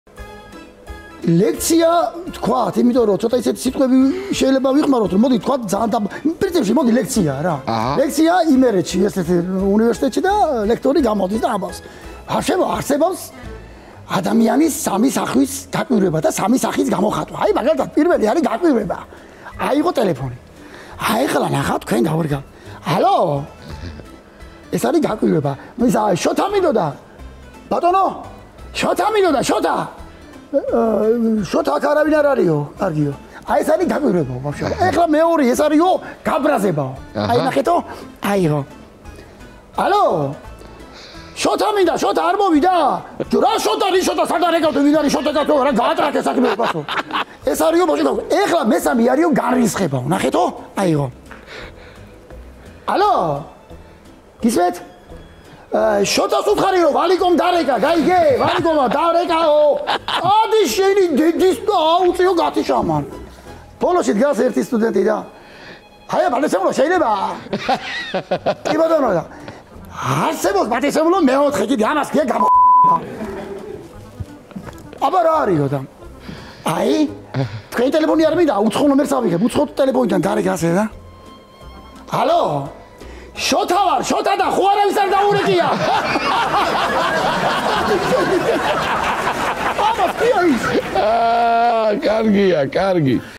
ლექცია იმერეთში-სკიპი-მამუკა ნადირაძე; ანეკდოტების შოუ-2024.02.04; Paata guliashvili
სკიპი-მამუკა ნადირაძე და გოგი გოგალაძე;

ანეკდოტების შოუ პაატა გულიაშვილთან ერთად-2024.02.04; Paata guliashvili
👉 გამოიწერეთ არხი, მოიწონეთ და გააზიარეთ ვიდეო.
პაატა გულიაშვილი-"შამო და იხახანე"
ანაეკდოების შოუ
პაატა გულიაშვილის შოუ